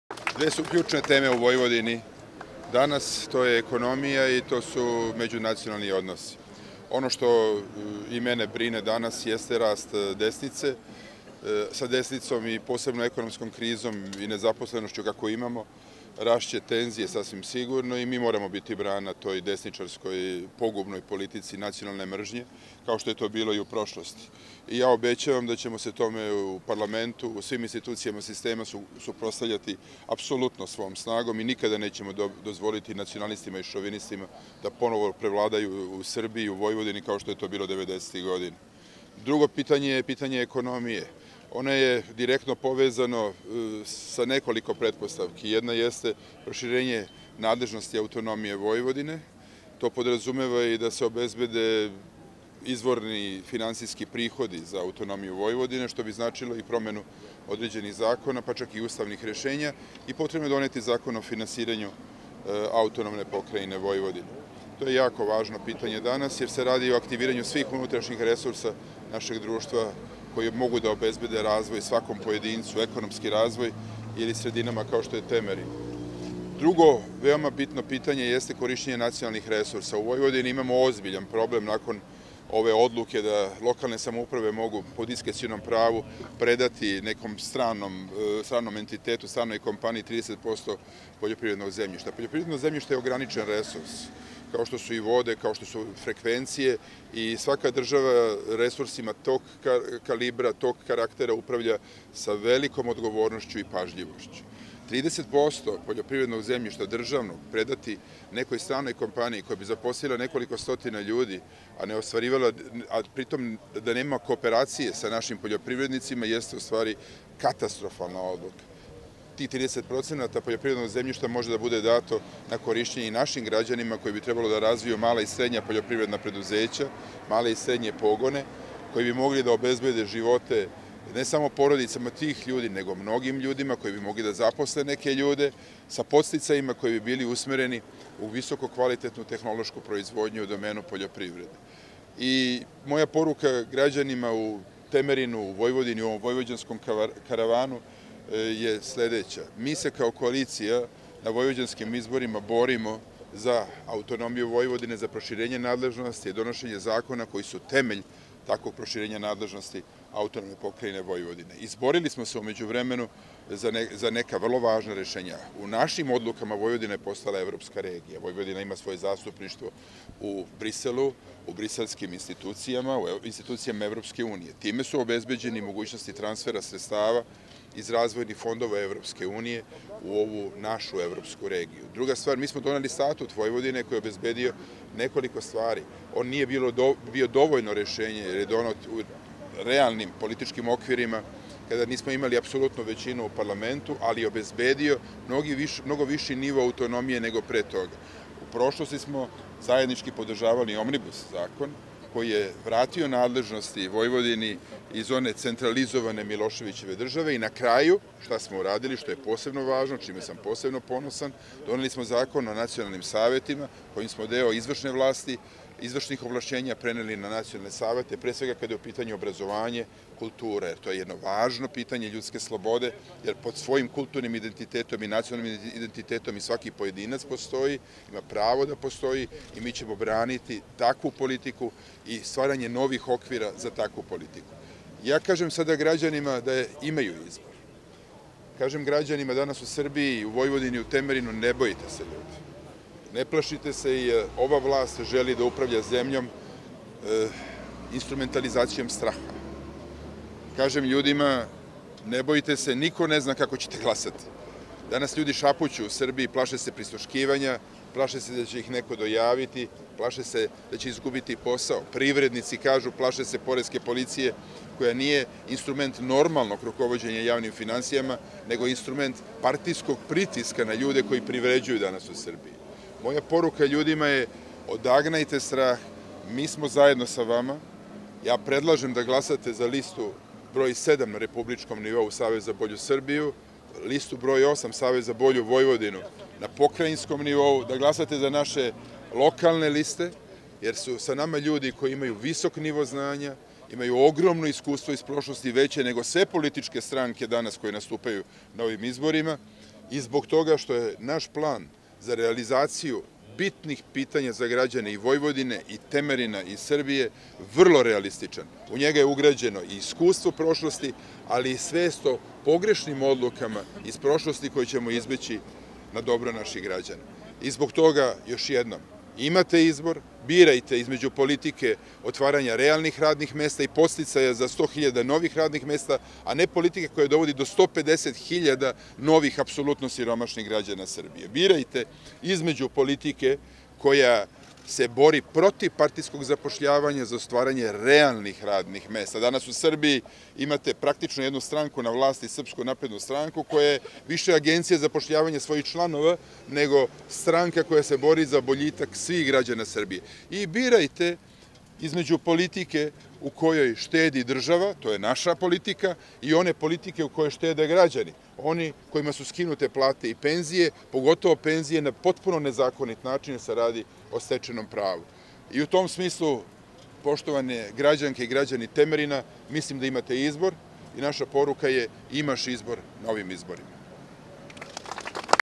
Izjava Borisa Tadića (SDS) u Temerinu (09.04.2016.)
Predsednik SDS Boris Tadić u Temerinu.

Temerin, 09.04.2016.